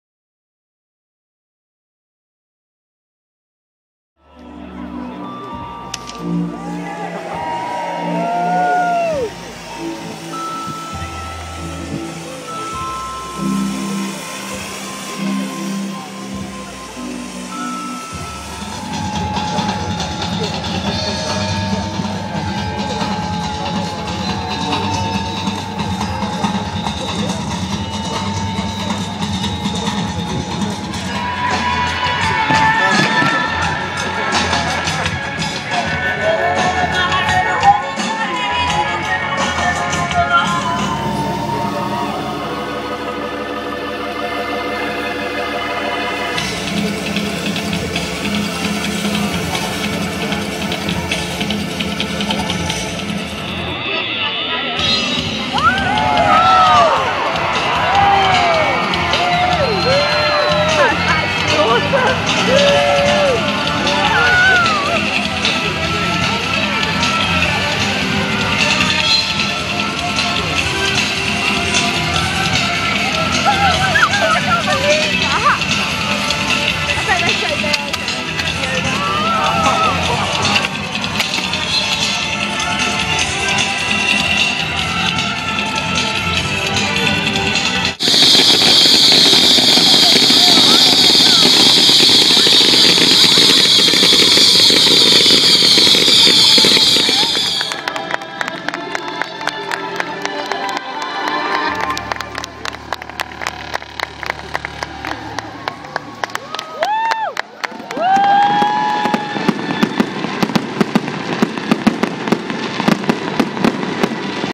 Exeter Fringe Festival 2011
Pyrotechnic water-born performance of 'Wishful' by Emergency Exit Arts, a London based company.

Midsummer party on the quay 2011
Opening night performance for the Exeter Fringe Festival.